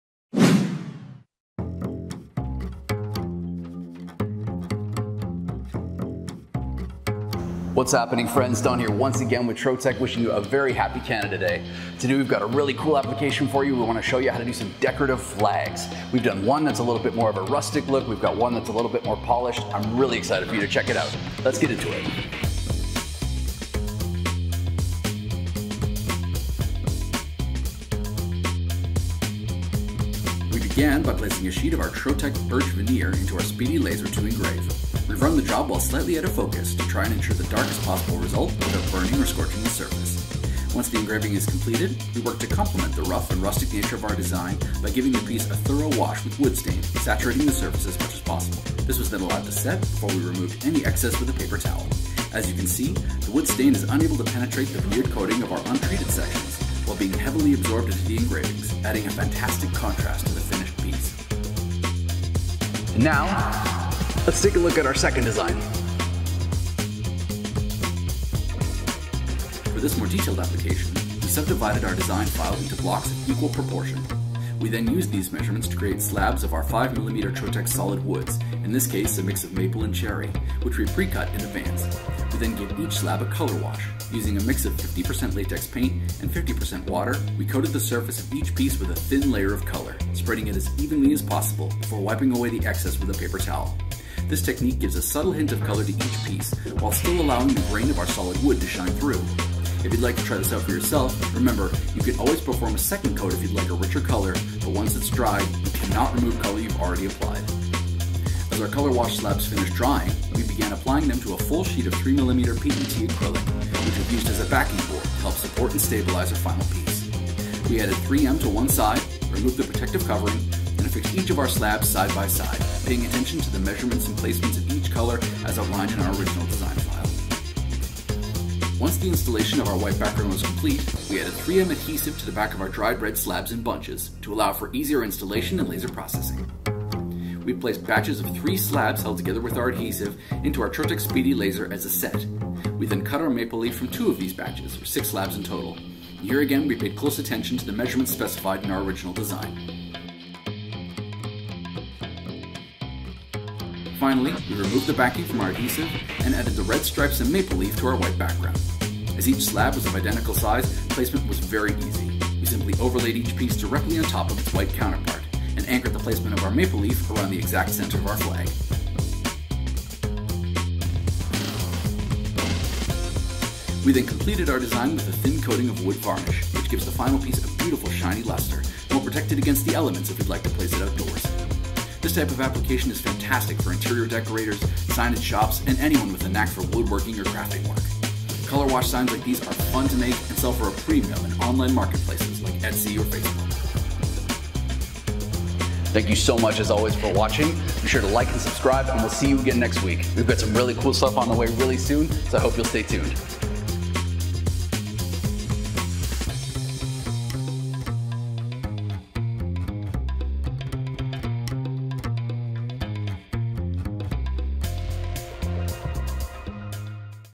Decorative Laser Cut Flags | Custom Laser Signage | Trotec Laser Tutorial - YouTube
In this Trotec laser tutorial, Don shows us how to create decorative laser cut flags and custom laser signage using Trotec's birch veneer and solid woods. Laser cut flags are a great way to add a personal touch to your space and make a statement. Don demonstrates two different techniques for creating flags with unique designs, one rustic and the other more polished.

The first technique involves engraving a sheet of Trotec's birch veneer using the speedy laser. Don runs the job while slightly out of focus to ensure the darkest possible result without burning or scorching the surface. He then complements the rough and rustic nature of the design by giving the piece a thorough wash with wood stain, which saturates the surface as much as possible. Once the wood stain sets, Don removes any excess with a paper towel. The wood stain is unable to penetrate the veneered coating of the untreated sections while being heavily absorbed into the engravings, adding a fantastic contrast to the finished piece.

The second technique involves subdividing the design file into blocks of equal proportion, and using these measurements to create slabs of Trotec's five millimeter solid woods, a mix of maple and cherry. Don then gives each slab a color wash using a mix of 50% latex paint and 50% water, coating the surface of each piece with a thin layer of color, spreading it as evenly as possible before wiping away the excess with a paper towel. This technique gives a subtle hint of color to each piece while still allowing the grain of the solid wood to shine through. Don advises that a second coat can be performed if a richer color is desired, but once it's dry, the color cannot be removed.

As the color wash slabs finish drying, Don begins applying them to a full sheet of three millimeter pt acrylic, which is used as a backing board to help support and stabilize the final piece. He adds 3M adhesive to one side, removes the protective covering, and affixes each of the slabs side by side, paying attention to the measurements and placements of each color as outlined in the original design. Once the installation of the white background is complete, Don adds 3M adhesive to the back of the dried red slabs and bunches to allow for easier installation and laser processing. He then places patches of three slabs held together with adhesive into the Trotec speedy laser as a set, and cuts the maple leaf from two of these batches, for a total of six slabs. Here again, he pays close attention to the measurements specified in the original design.

Finally, Don removes the backing from the adhesive and adds the red stripes and maple leaf to the white background. As each slab is of identical size, placement is very easy. He simply overlaid each piece directly on top of its white counterpart and anchored the placement of the maple leaf around the exact center of the flag. Don completes the design with a thin coating of wood varnish, which gives the final piece a beautiful shiny luster and will protect it against the elements if it's placed outdoors.

This type of application is fantastic for interior decorators, signage shops, and anyone with a knack for woodworking or crafting. Color wash signs like these are fun to make and sell for a premium in online marketplaces like Etsy or Facebook. Join us for this fun and educational tutorial and learn how to create your own custom laser signage and decorative flags with Trotec! Don is an expert in his field and will share valuable tips and tricks to ensure that your finished product looks amazing.